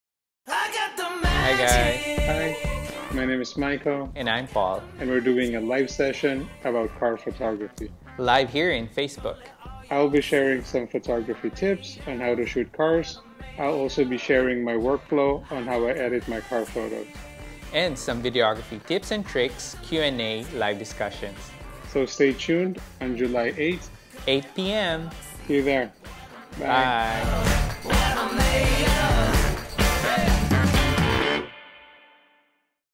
Mike & Paul Facebook Live Session on Car Photography on July 8, 8pm Dubai
Michael R. Cruz
Paul Aggarao